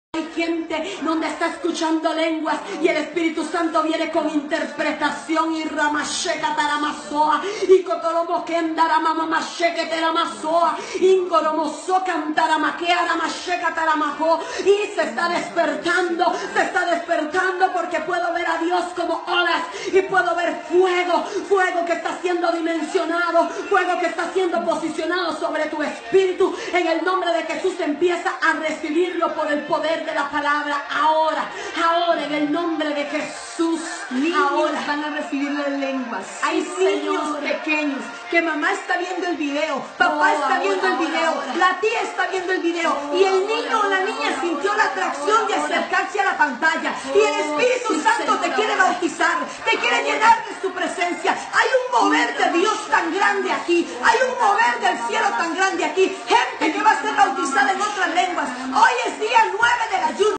Vídeo de esposa de aspirante presidencial en Costa Rica se volvió viral en redes sociales
La publicación que data del 10 de enero de este año, se puede observar a Laura Moscoa, esposa del candidato presidencial por el Partido Restauración Nacional, Fabricio Alvarado hablar en lenguas, algo que ha generado diversas reacciones en redes sociales a pocos días de las elecciones generales en este país centroamericano.

Redacción CB24